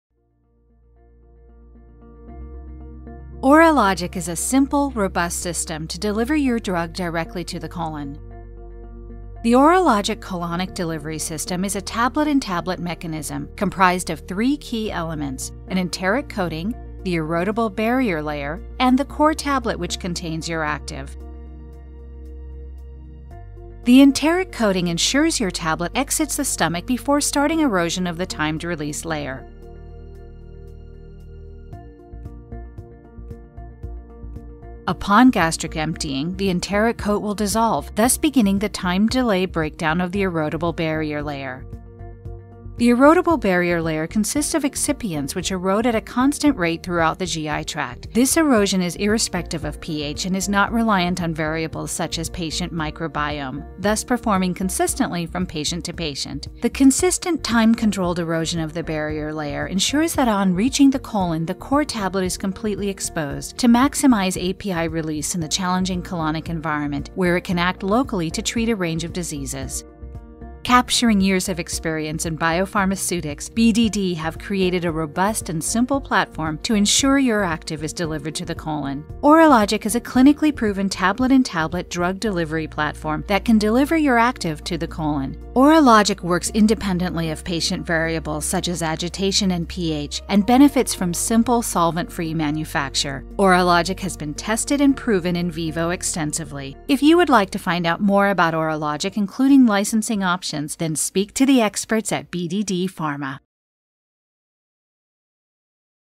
A tablet that delivers drug to the colon - CologiK™
Targeting Colon Delivery with CologiK™
BDD's patent protected tablet: CologiK™ is a timed-release technology; enabling consistent pH-independent delivery of drugs targeted to the colon.

Exploiting the well documented transit times through the GI tract, OralogiK™ tablets can be designed to release the drug at a target time after dosing which will promote release in the colon.

OralogiK™ is a tablet-in-tablet technology in which an erodible barrier layer is compressed around a drug containing core tablet. The barrier layer erodes at a constant rate throughout the GI tract, unaffected by variables such as agitation and pH until the core tablet becomes exposed and the contents are released.

The erosion mechanism ensures that at the point of release, the barrier layer is no longer present and will not impede release from the core. This is important for facilitating complete release from the tablet core in the colonic environment where there is minimal agitation and only small pockets of fluid.

The OralogiK™ platform has been designed to work independently of GI tract variables such as agitation, pH and microbiome which is critical for consistent performance in healthy and diseased populations.